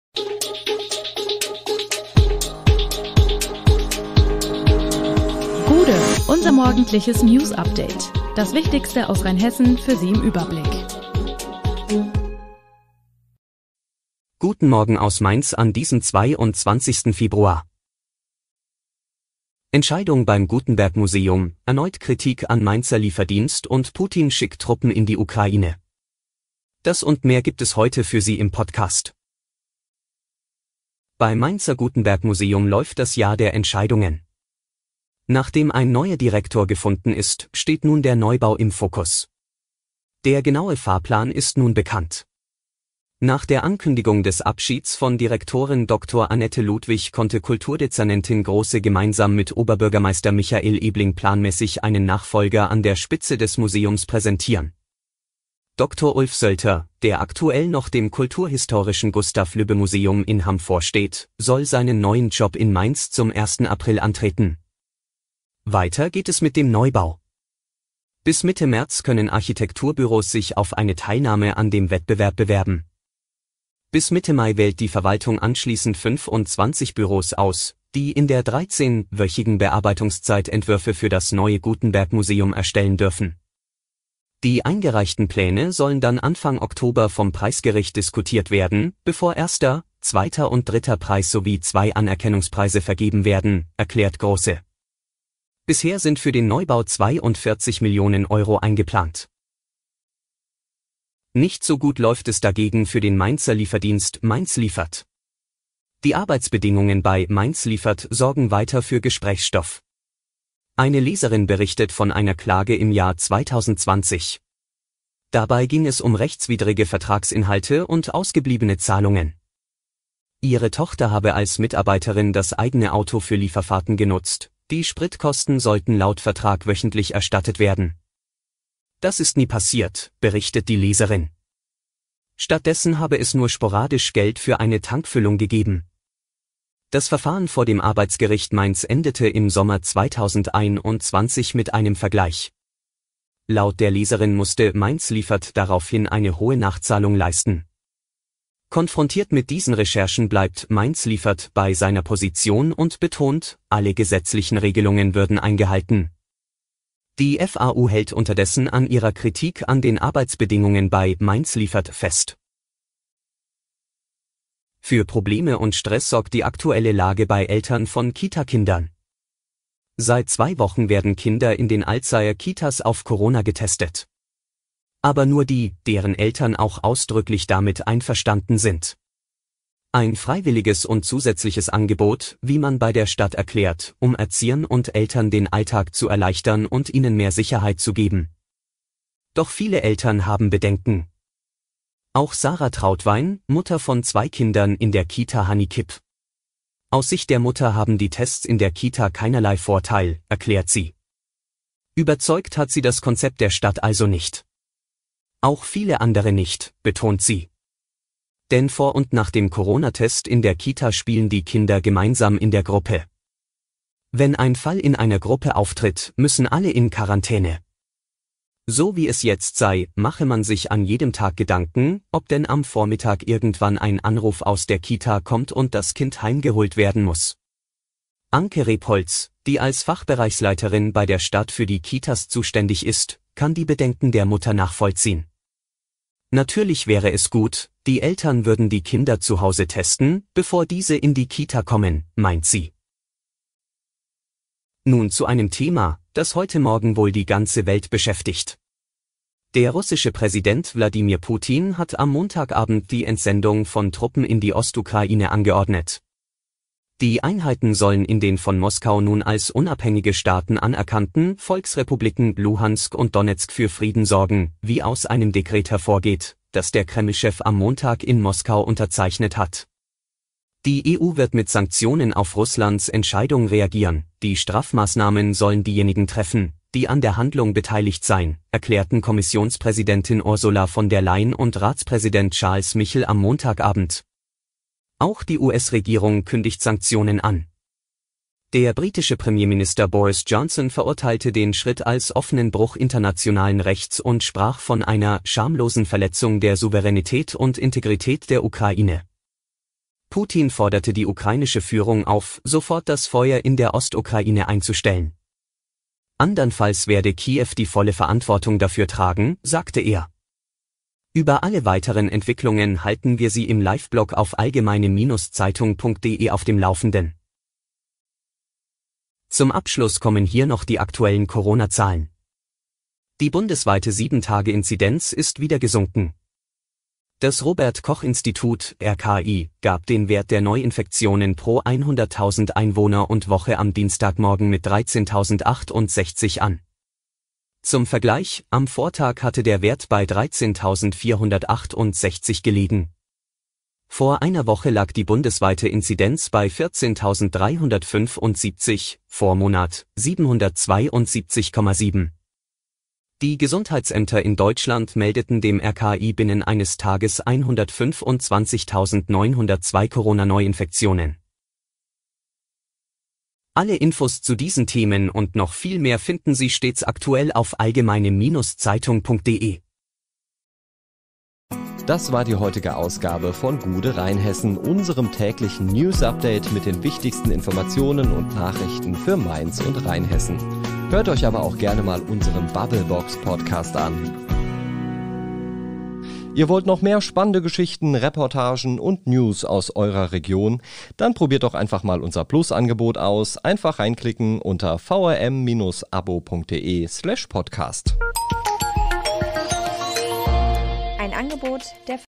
Jahr der Entscheidungen beim Gutenberg-Museum - Gude, Rheinhessen!
Entscheidung beim Gutenberg-Museum, erneut Kritik an Mainzer Lieferdienst und Putin schickt Truppen in die Ukraine. Alle Hintergründe zu den Nachrichten des Tages finden Sie hier: